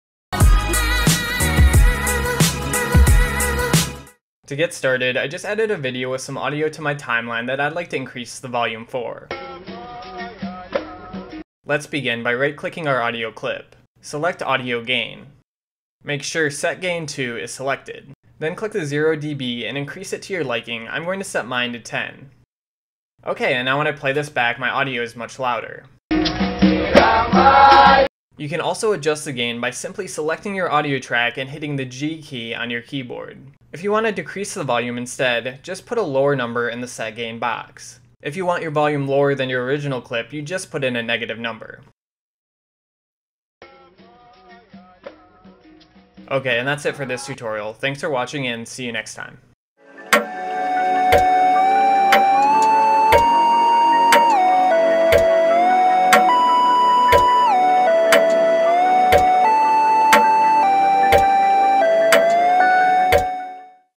How to Increase Audio Volume in Premiere Pro
A tutorial on how to increase audio volume levels in premiere pro.